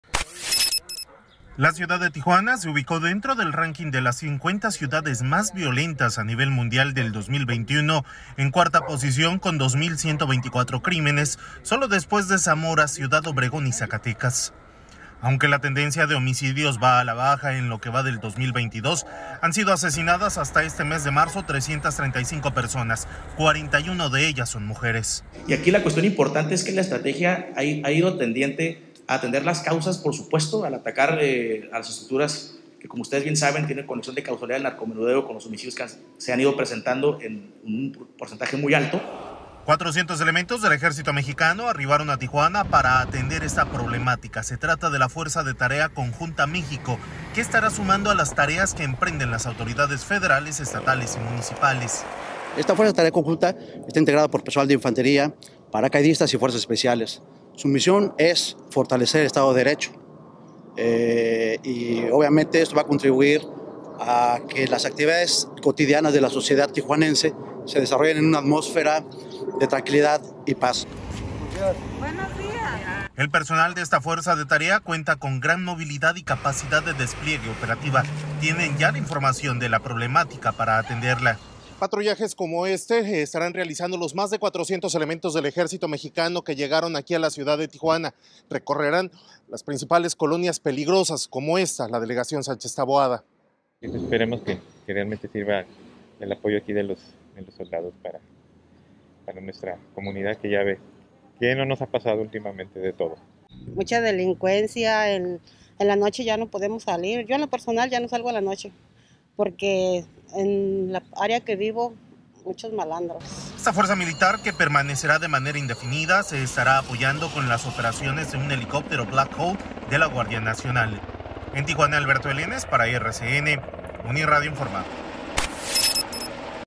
Llegan 400 militares a Tijuana para contención de homicidios
Aunque la tendencia de homicidios va a la baja, en lo que va del 2022, han sido asesinadas, hasta este mes de marzo, 335 personas, 41 de ellas son mujeres.